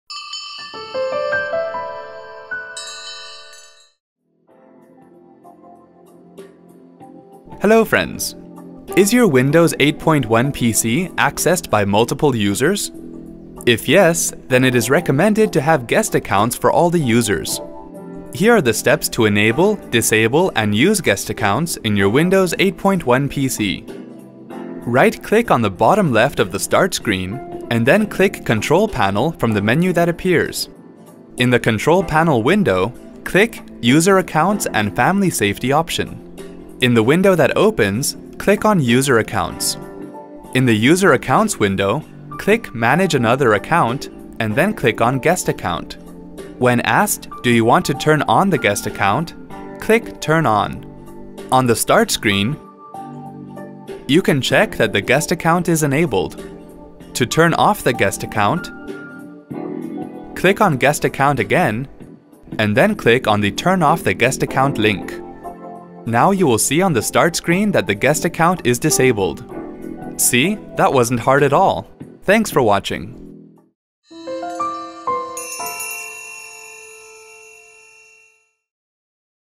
How to Enable/Disable and use the Guest account in Windows® 8.1
A guest account allows people who don't have a permanent account to use your computer without having access to your personal files. People using the guest account can't install software or hardware, change settings, or create a password. If you wish to enable, disable and use the Guest Account in Windows® 8.1, then follow the easy steps shown in this video.